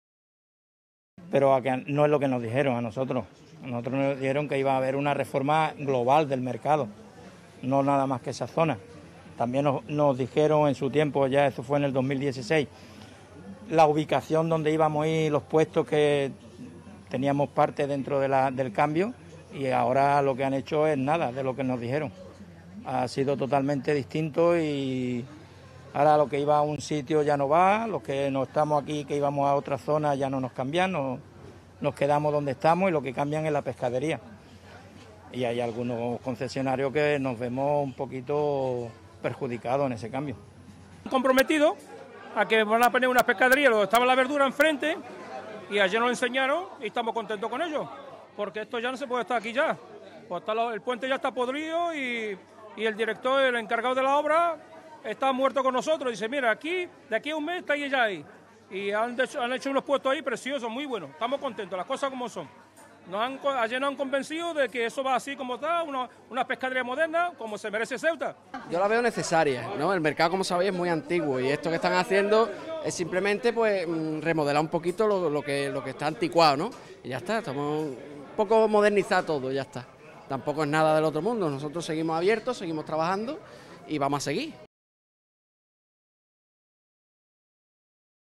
División de opiniones en los concesionarios del mercado
Entre los concesionarios del Mercado parece haber división de opiniones. Mientras que los profesionales de pescadería están de acuerdo con las obras urgentes y con su cambio de ubicación, otros concesionarios no están tan conformes.